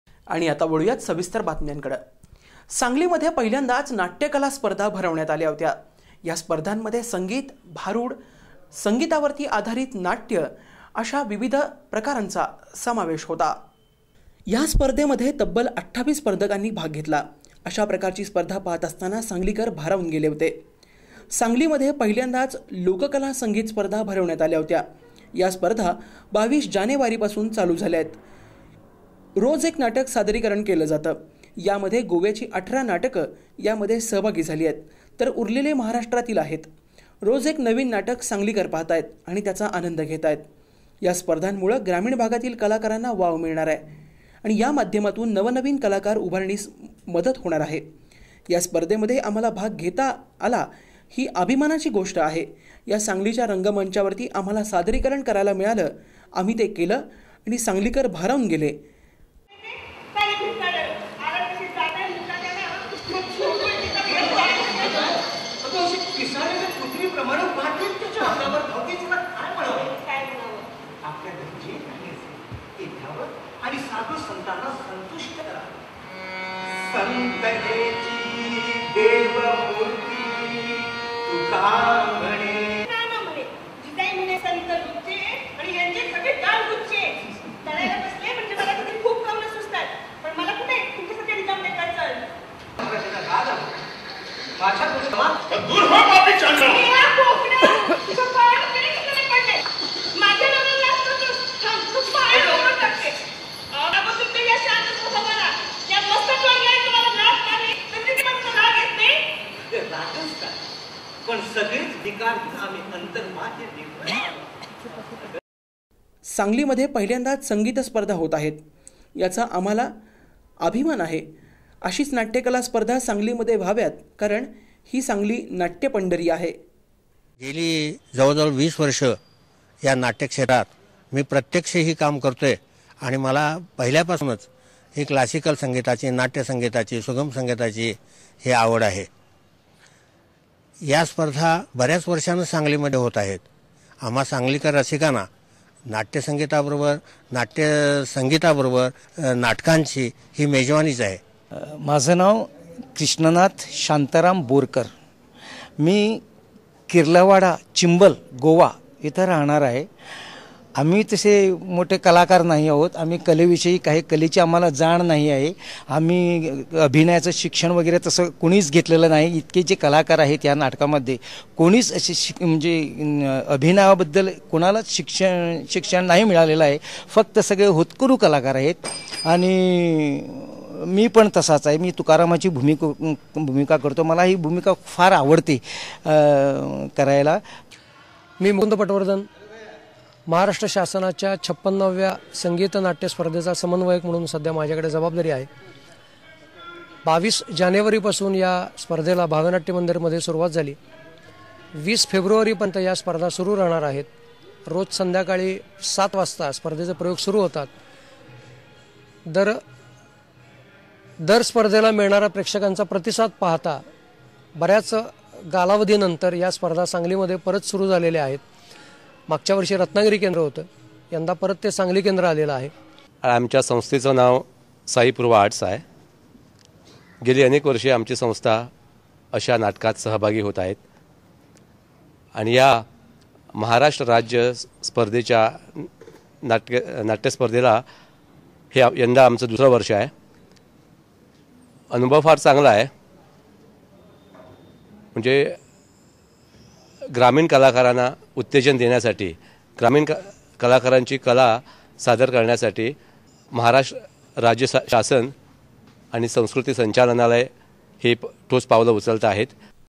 सांगली :- सांगली मध्ये पहिल्यांदाच भरवण्यात आल्या नाट्य कला स्पर्धा,भारूड, संगीत,लोक संगीतावर सांगली
सांगली :- सांगली मध्ये पहिल्यांदाच भरवण्यात आल्या नाट्य कला स्पर्धा,भारूड, संगीत,लोक संगीतावर सांगलीकर गेले भारावून.पाहा फक्त SBN मराठी.